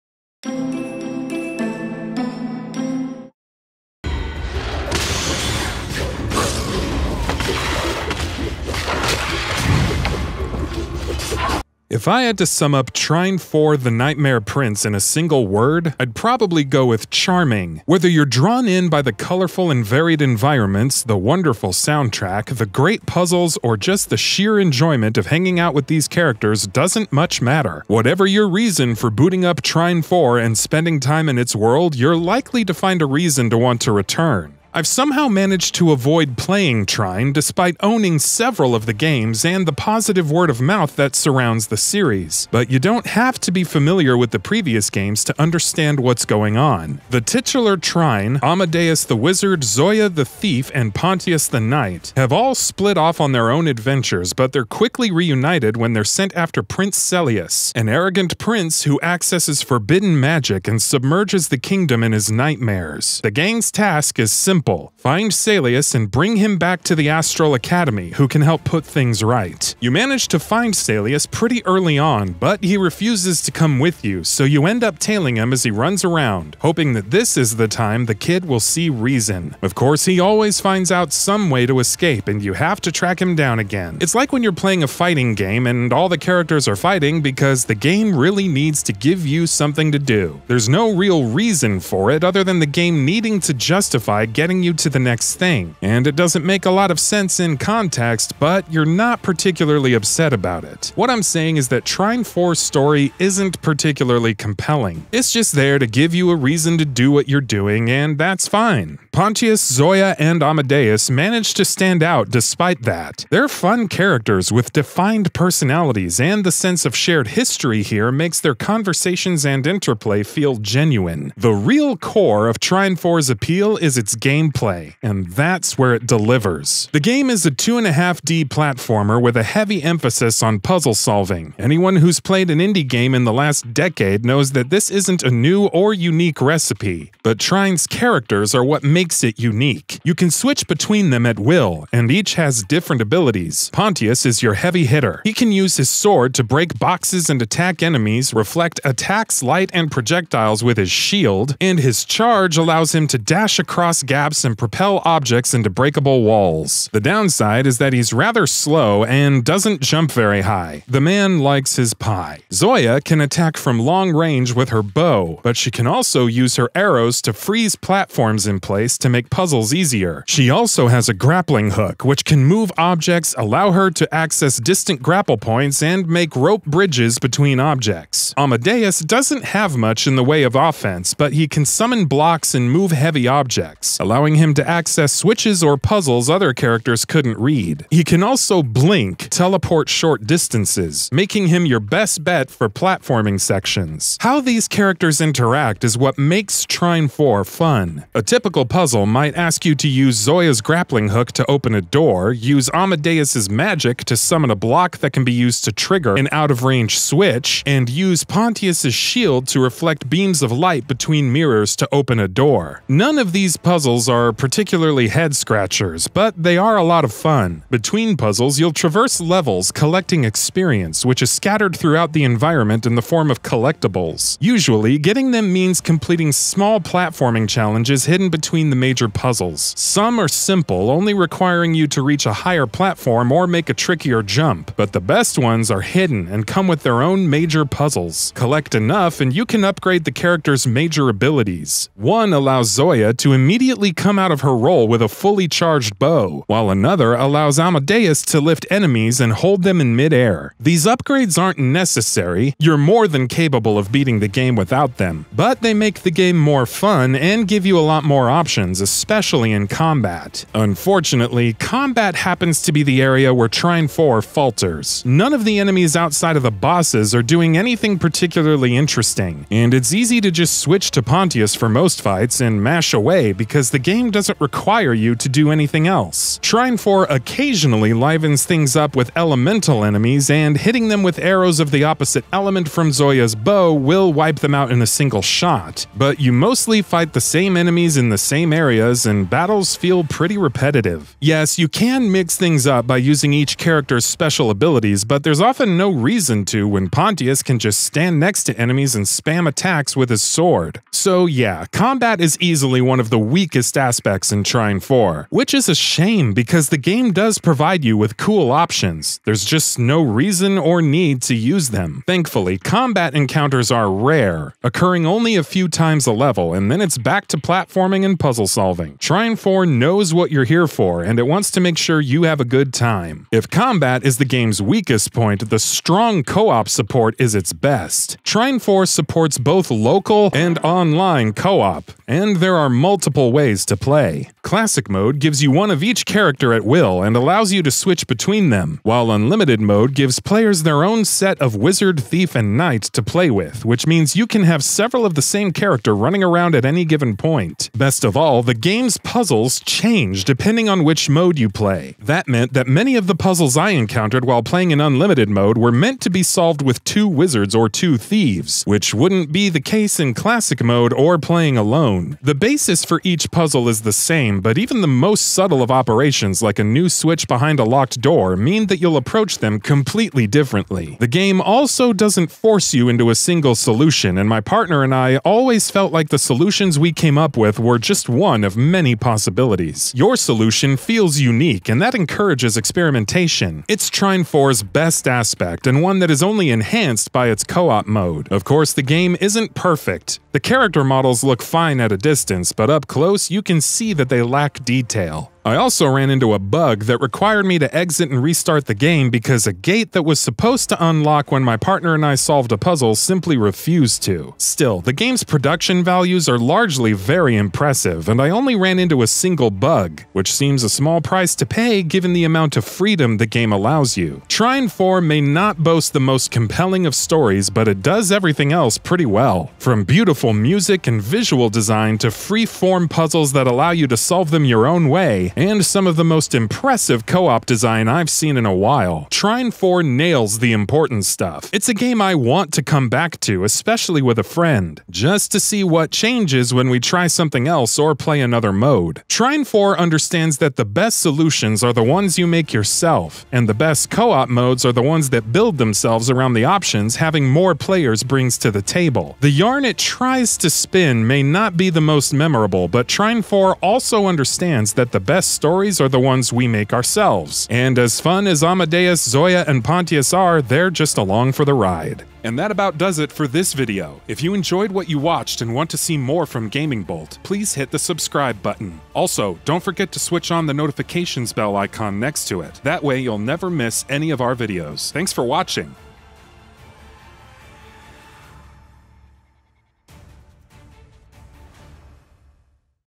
Trine 4: The Nightmare Prince Review - The Final Verdict
It may not spin the most memorable yarn and have the odd bug, but Trine 4 is an excellent co-op game that understands that the best puzzle solutions and the ones you make yourself, and the best co-op modes are the ones that build themselves around the options having more players brings to the table.